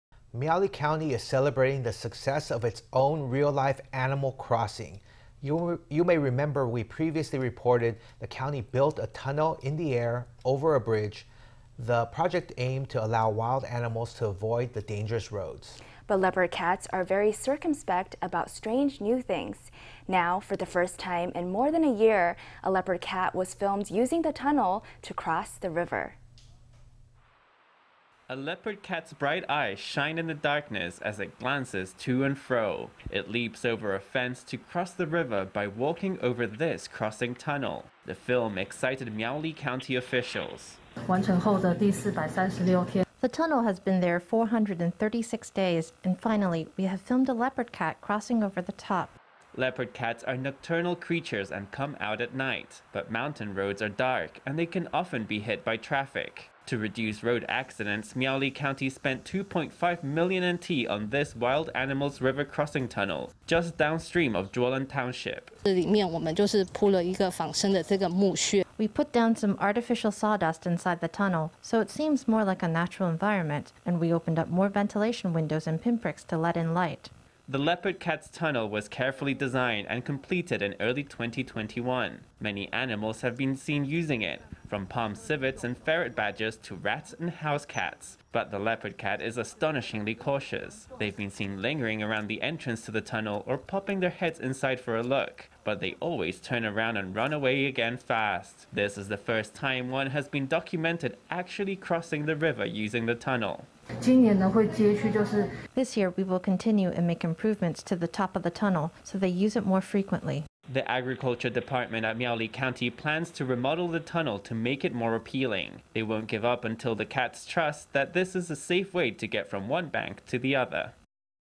Miaoli wild animals’ crossing is favored by its first-ever leopard cat
Miaoli County is celebrating the success of its own real-life “Animal Crossing”! You may remember we previously reported the county built a tunnel in the air over a river. The project aimed to allow wild animals to avoid the dangerous roads. But leopard cats are very circumspect about strange new things. Now, for the first time in more than a year, a leopard cat was filmed using the tunnel to cross the river.

A leopard cat’s bright eyes shine in the darkness as it glances to and fro. It leaps over a fence to cross the river by walking over this crossing tunnel. The film excited Miaoli County officials.

Chang Wei
Miaoli County Agriculture Dept.
The tunnel has been there 436 days, and finally we have filmed a leopard cat crossing over the top.

Leopard cats are nocturnal creatures and come out at night. But mountain roads are dark and they can often be hit by traffic. To reduce road accidents, Miaoli County spent NT$2.5 million on this “Wild Animals’ River Crossing Tunnel,” just downstream of Zhuolan Township.

Chang Wei
We put down some artificial sawdust inside the tunnel, so it seems more like a natural environment, and we opened up more ventilation windows and pinpricks to let in light.

The leopard cats’ tunnel was carefully designed and completed in early 2021. Many animals have been seen using it, from palm civets and ferret badgers to rats and house cats. But the leopard cat is astonishingly cautious. They’ve been seen lingering around the entrance to the tunnel, or popping their heads inside for a look. But they always turn around and run away again fast. This is the first time one has been documented actually crossing the river using the tunnel.

Chang Wei
This year we will continue and make improvements to the top of the tunnel, so they use it more frequently.

The Agriculture Department at Miaoli County plans to remodel the tunnel to make it more appealing. They won’t give up until the cats trust that this is a safe way to get from one bank to the other.